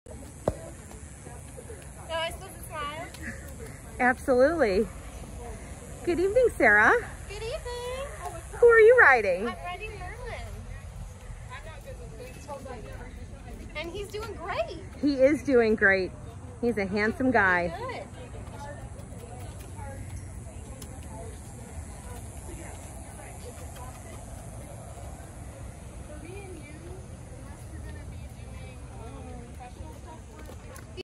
Sarah and Merlin the Wizard at Wildhorse Ranch Rescue
Sarah and Merlin the Wizard at Wildhorse Ranch Rescue ~  bareback and bitless. We've used Parelli Natural Horsemanship at our ranch since 1995.